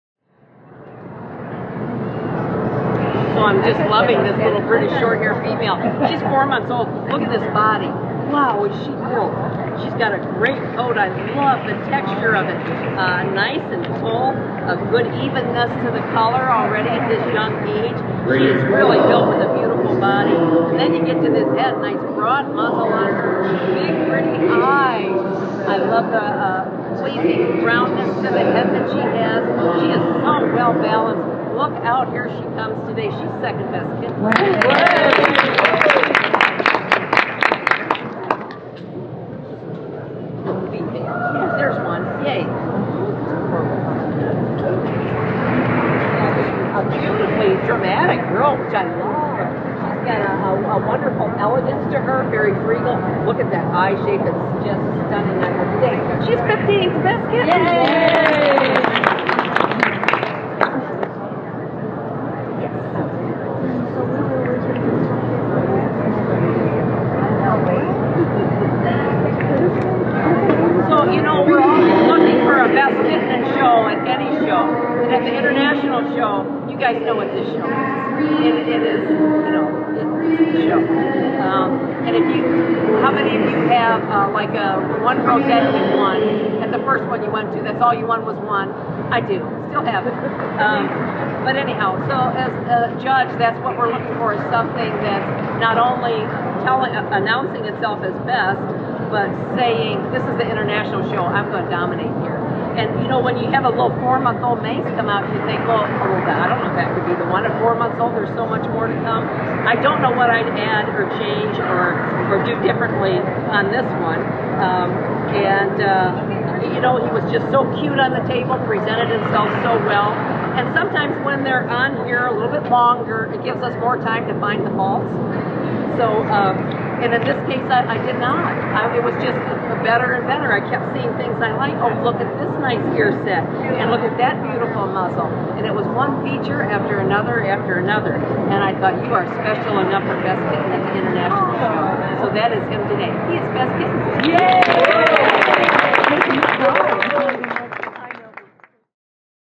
CFA International Show 2018 - Rachel Anger's Shorthair Kitten Final
Rachel Anger's Shorthair Kitten final at the 2018 CFA International Cat Show held in Cleveland, Ohio, USA.

  Well, as there were 15 kittens in the final, a small portion thereof. As previously noted, Rachel only took out her top cats, so otherwise there wasn't much to see other than a kitten in a cage and sometimes just whiskers, a foot  or no cat at all, just a cage. I thought my time would be better spent on cats you can see rather than those you can't - but I decided to show the top cats (mostly - she did this final out of order).